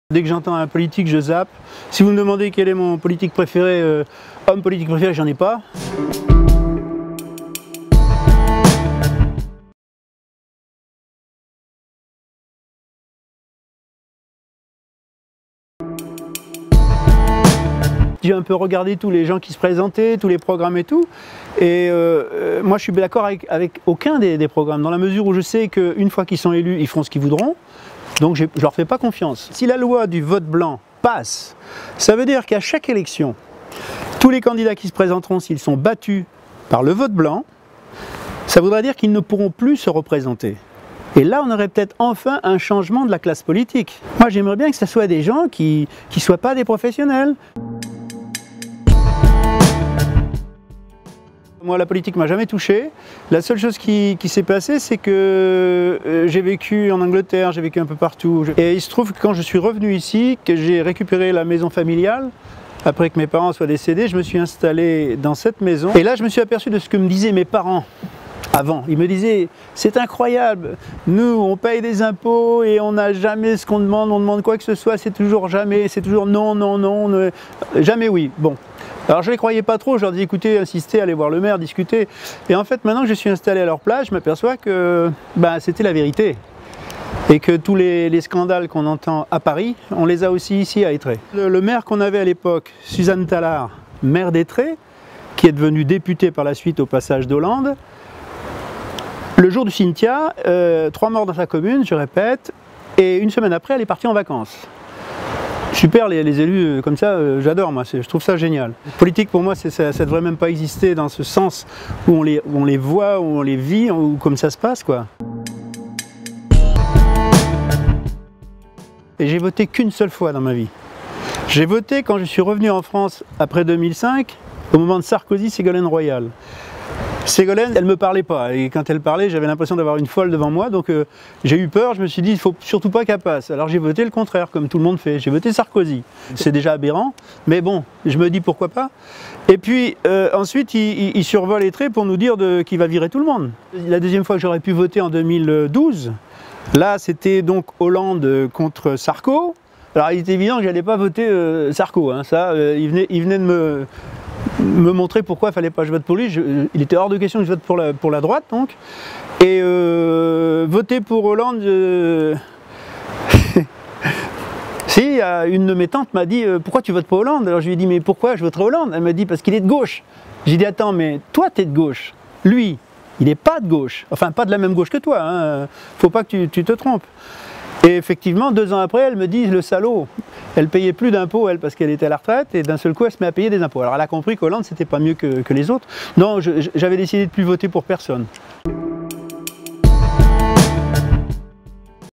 "Dès que j'entends un politique, je zappe" Guy, retraité, vote blanc
LA RAGE AU VENTRE, épisode 4. Guy, retraité de la production musicale, a parcouru le monde avant de regagner la maison familiale après le passage de la tempête Xynthia, à Aytré (17). Cette année, il a choisi de voter blanc. Celui qui préfère l'humain à la politique nous explique ses idées, son parcours, son point de rupture, qui l'ont amené à ce choix. C'est le quatrième volet de notre série "La rage au ventre".